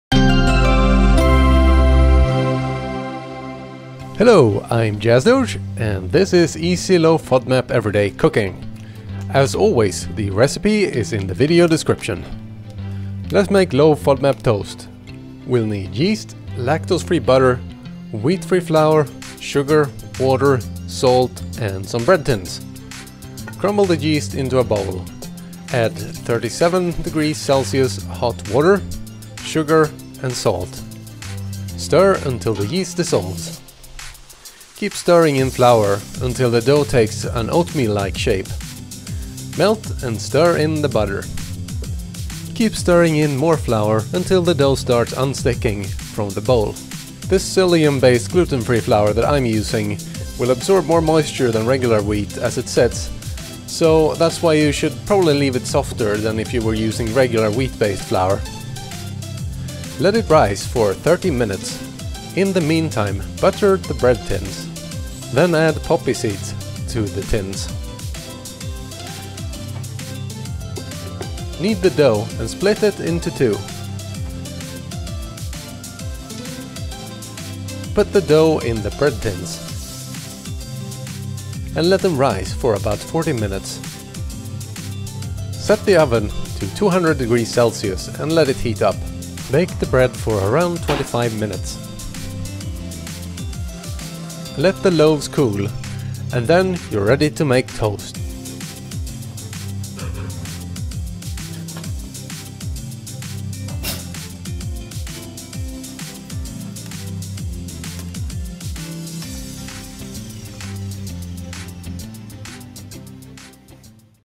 Jasdoge's Kitchen Episode 13 - Low FODMAP Toast Loaf!
YEAH TOAST! Here's how you can make low FODMAP toast loaves!

Jasdoge's Kitchen is a series of low FODMAP recipes designed to be quick and easy, for your everyday cooking.